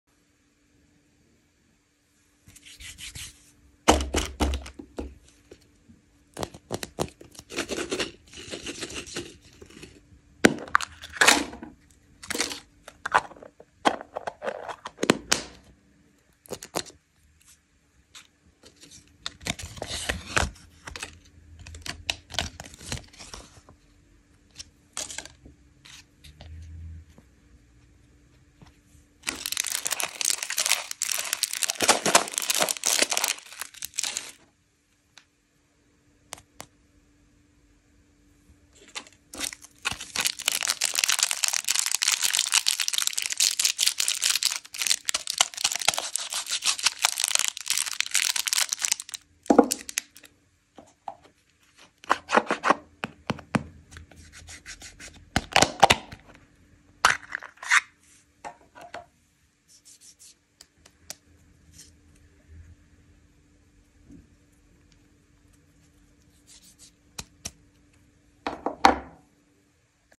Yummy Rainbow Lollipops Unpacking ASMR 🍭 _ Learn Colors with Lollipops and Sweets.
Yummy Rainbow Lollipops Unpacking ASMR 🍭 _ Learn Colors with Lollipops and Sweets.I love sweets, I hope you also love chocolate eggs or candy.